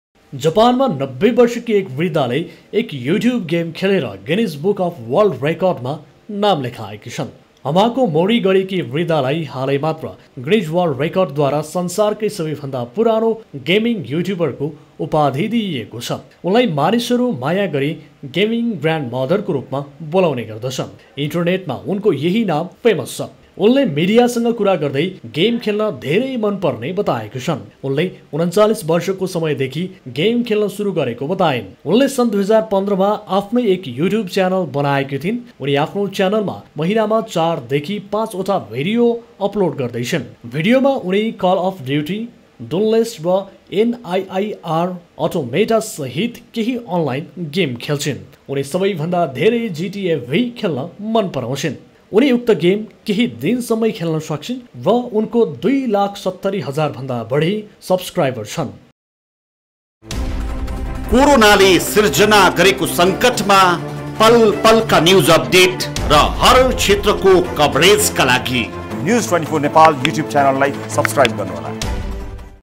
जापानकी ९० वर्षीया बृद्धले युट्युबमा गेम खेलेर गिनिज बुकमा नाम लेखाइन् - NEWS24 TV
जापानमा ९० वर्षकी एक वृद्धाले एक युट्युब गेम खेलेर गिनिज बुक अफवल्र्ड रेकर्डमा नाम लेखाएकी छन् । हमाको मोरी गरेकी यी बृद्धालाई हालै मात्र गिनिज वर्ल्ड रेकर्डद्वारा संसारकै सबैभन्दा पुरानो गेमिंग युट्युबरको उपाधि दिइएको छ । उनलाई मानिसहरु माया गरी गेमिंग ग्रान्डमदरको रुपमा बोलाउने गर्दछन् । इन्टरनेटमा उनको यहि नाम फेमस छ । उनले मिडियासँग कुरा गर्दै गेम खेल्न धेरै मन पर्ने बताएकी छन् । उनले ३९ वर्षको समय देखि गेम खेल्न शुरु गरेको बताइन् । उनले सन् २०१५ मा आफ्नै एक युट्युब च्यानल बनाएकी थिइन् । उनी आफ्नो च्यानलमा महिनामा ४ देखि ५ वटा भिडियो अपलोड गर्दैछिन् । भिडियोमा उनी कल अफ ड्युटी, डुनलेस र एनआईईआर अटोमेटा सहित केहि अनलाईन गेम खेल्छिन् । उनी सबैभन्दा धेरै जीटीएभी खेल्न मन पराउँछिन् । उनी उक्त गेम केहि दिनसम्मै खेल्न सक्छिन् ।